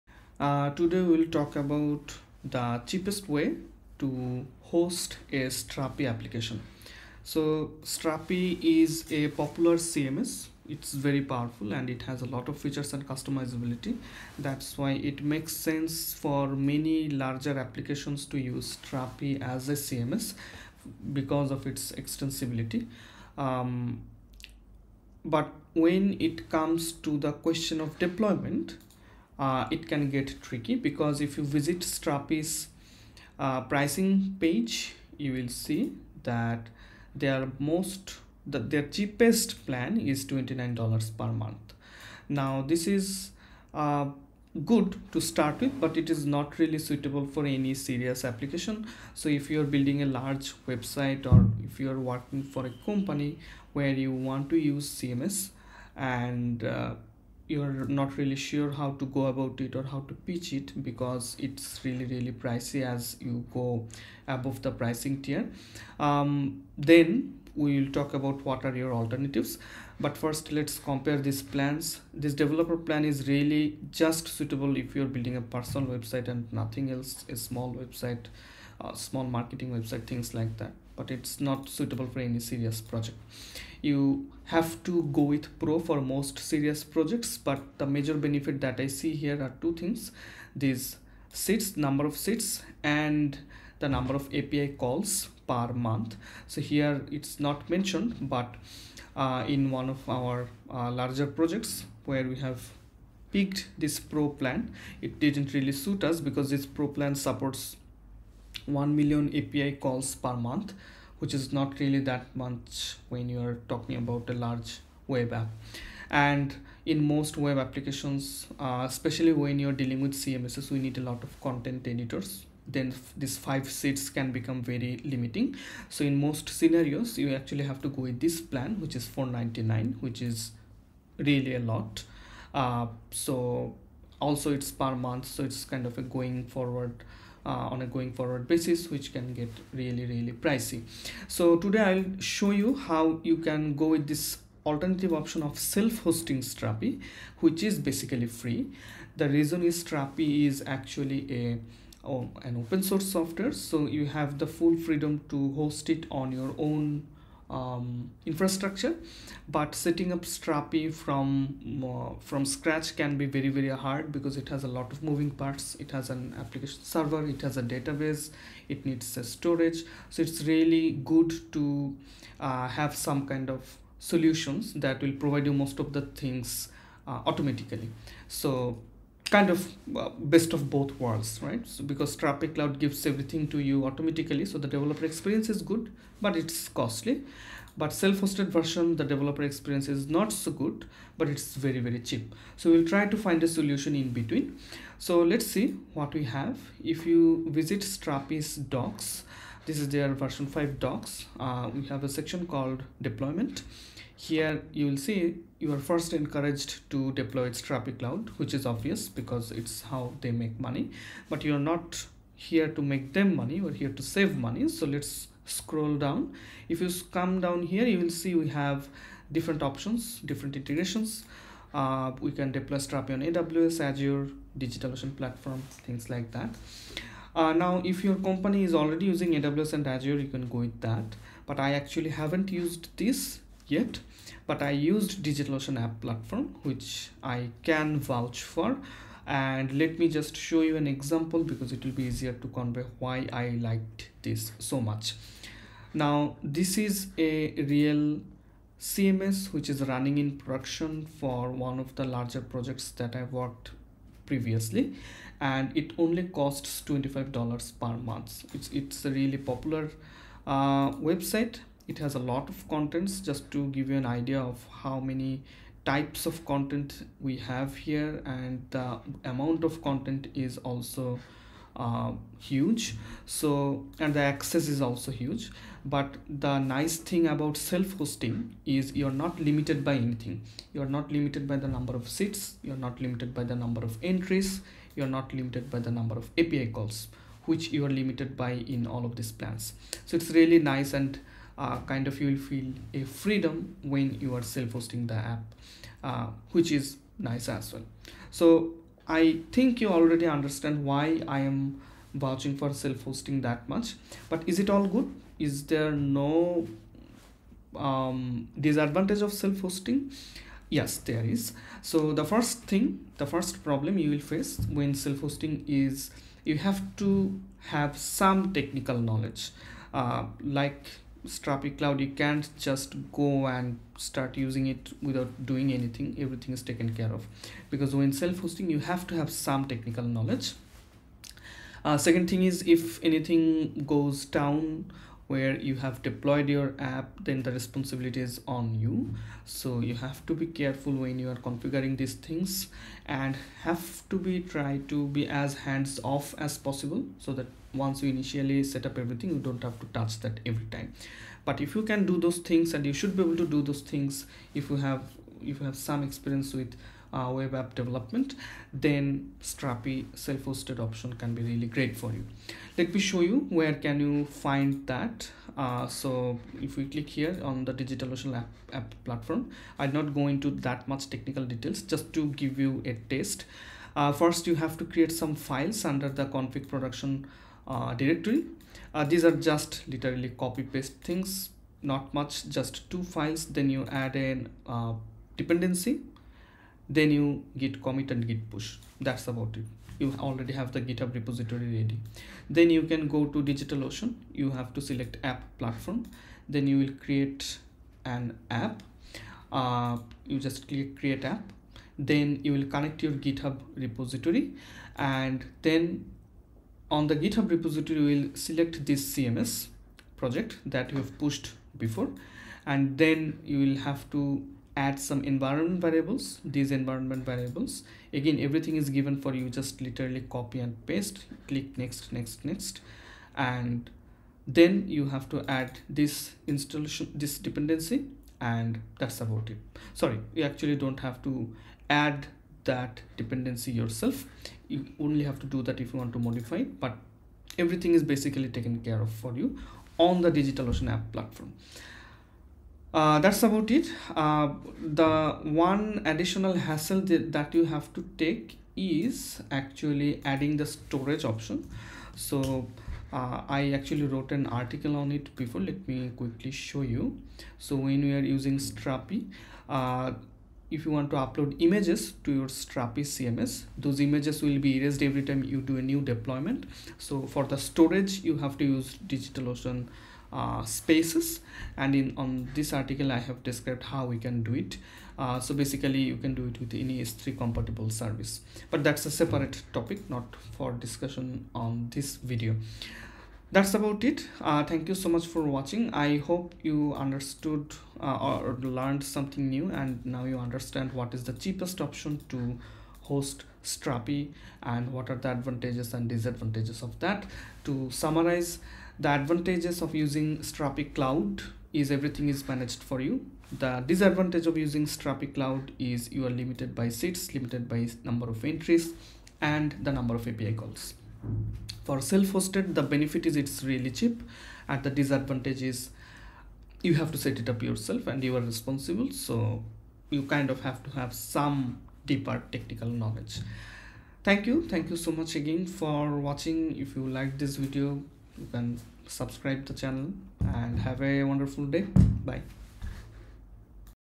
Cheapest way to host Strapi CMS
In this video we talk about hosting Strapi CMS in digital ocean. This is by far the cheapest way to host the CMS for me.